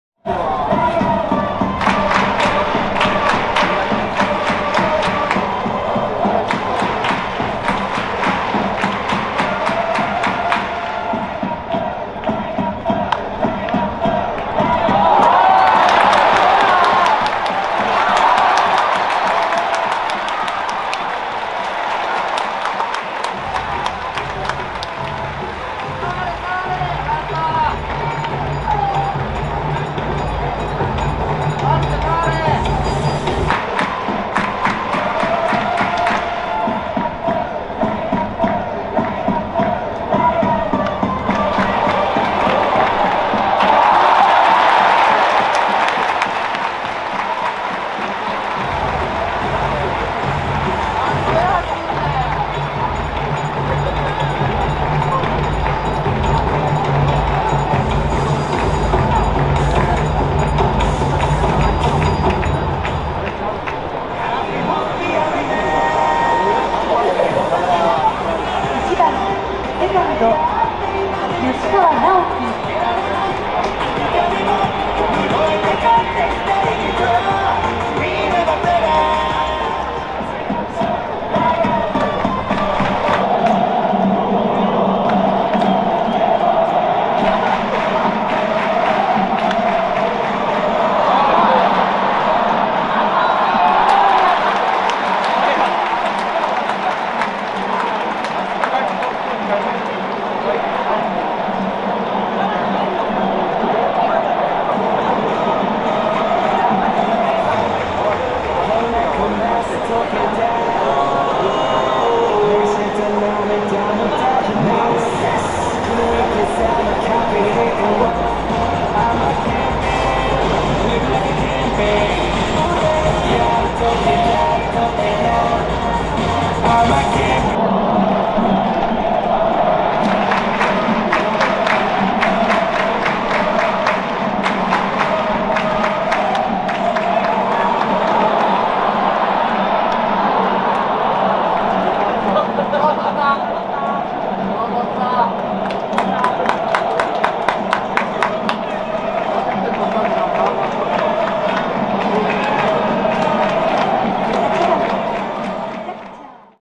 1試合4エラー 阪神タイガース マルテ選手 投手西勇輝 対読売ジャイアンツ 20201023東京ドームご覧頂いた方々是非チャンネル登録お願いいたします！200人突破！
林晃太選手　円陣をまかせられる